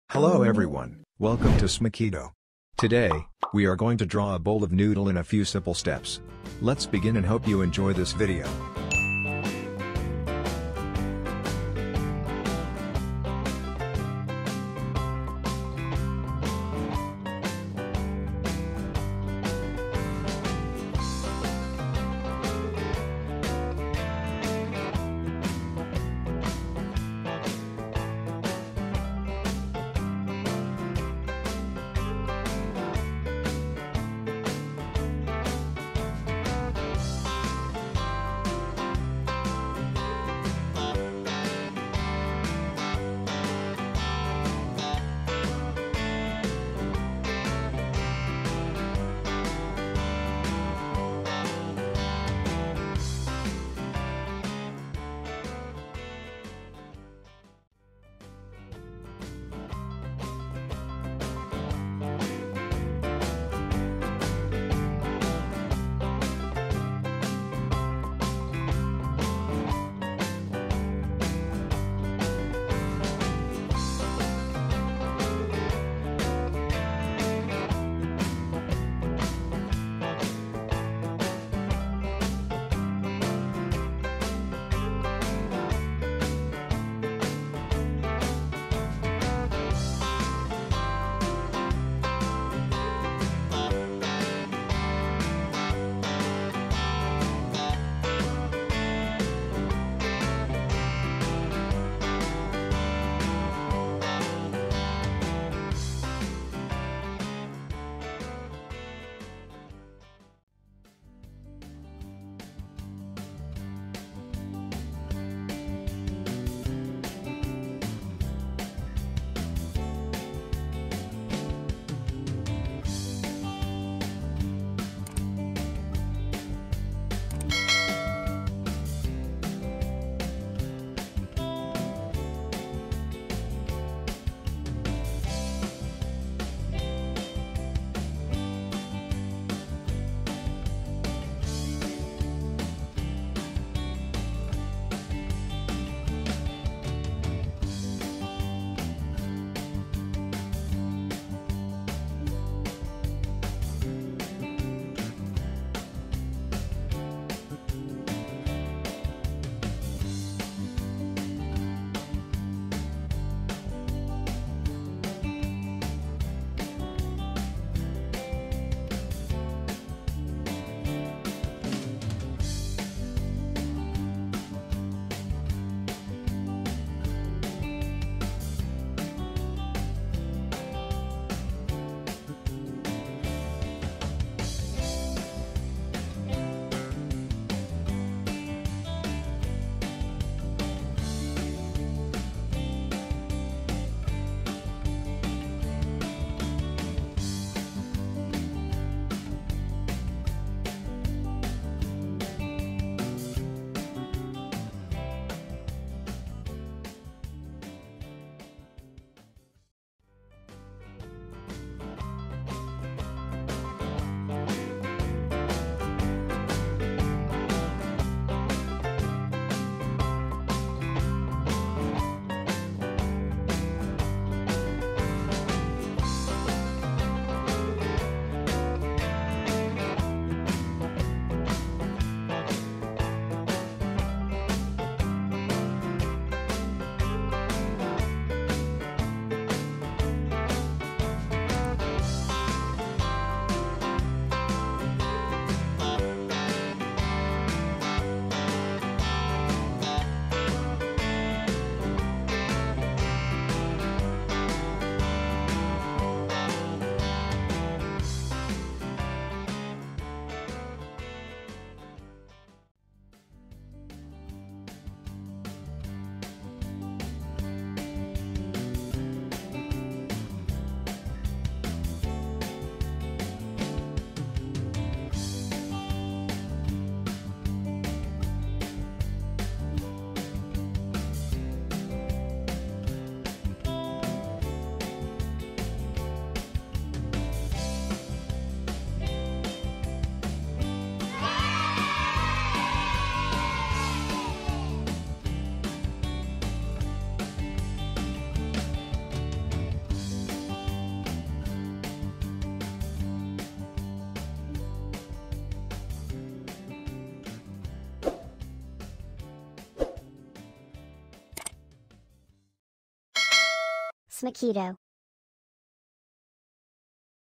How To Draw a Bowl of Noodle | Easy Noodle Drawing For Kids Step by Step Tutorial
How to draw a Bowl of Noddle step by step for kids and beginners.
In this video, you will learn the basic steps to draw a bowl of noodle.

Tools required:
1. Normal black marker pen or pencil
2. A4/ A5 white paper
3. Coloring pens/ Marker pen/ Coloring tools

✔️Drawing
✔️How to Draw
✔️Cartoon Noodle Drawing
✔️How to Draw a Bowl of Noodle
✔️Noodle Drawing Easy Steps
✔️Noodle Drawing For Kids
✔️Simple Noodle Drawing